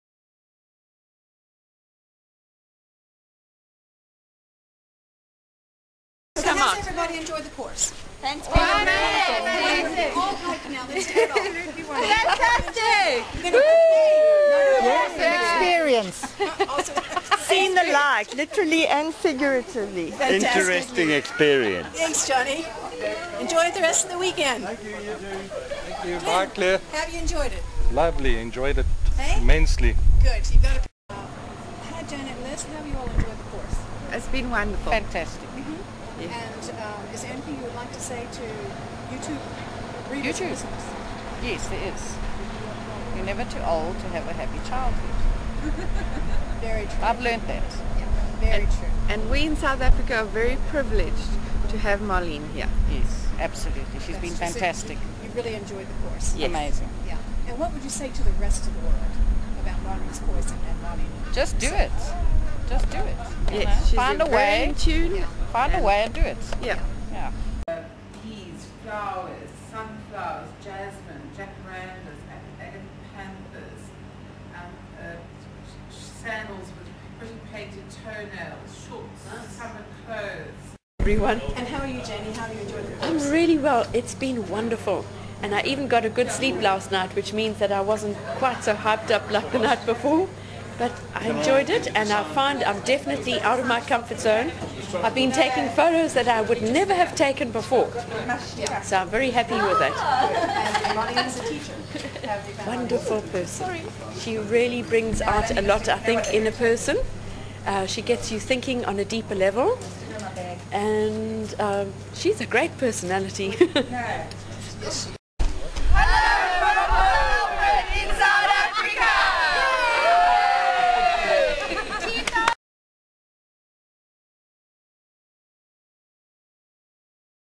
CREATIVITY THROUGH PHOTOGRAPHY COURSE - MARLENE NEUMANN - PORT ALFRED Video 2
One of Marlene's National Creativity through Photography Workshops was recently held in Port Alfred... students from all over the South Africa arrived at the My Pond Hotel to experience one of her inspirational Marlene Neumann Workshops...